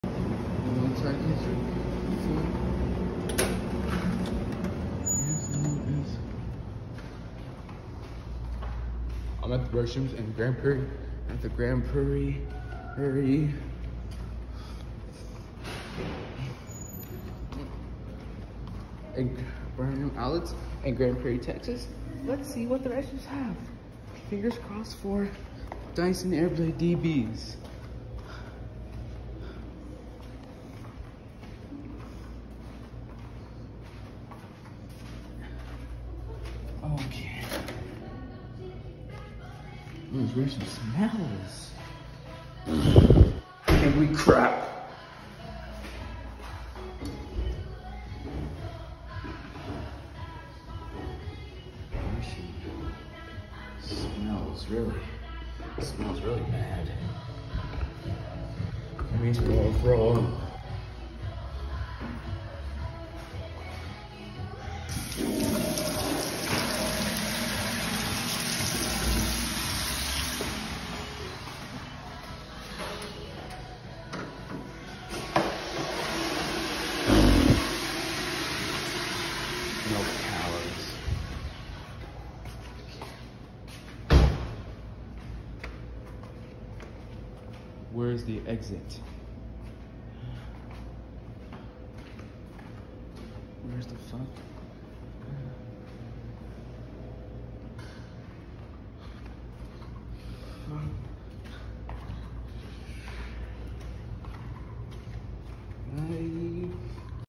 Paper Cowards Fail Grand Prairie Premium Outlets Grand Prairie TX
WHY THE F ING HELL CAN’T YA’LL INSTALL LIKE DYSON AIRBLADE DBS OR LIKE A COMBO OF DRYERS?!